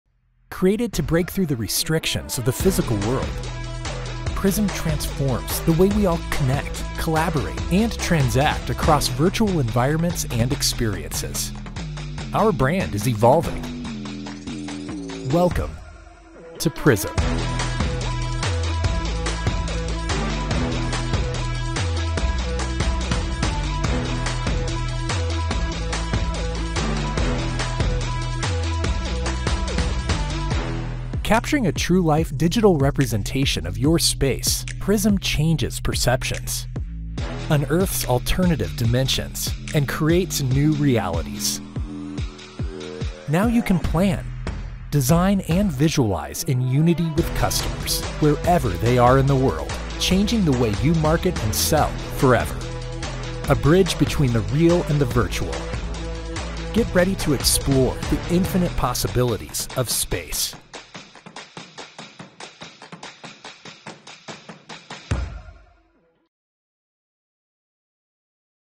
Welcome to Prismm - The Next Generation of Allseated!
Prismm captures a true life digital representation (a digital twin) of physical environments with an immersive spatial technology platform that transcends the limitations of a real space. Able to show the full spectrum of potential layouts, organizations can edit the digital twin to match the customers requirements exactly. Businesses can now plan, design and visualize spaces in harmony with
customers — wherever they are.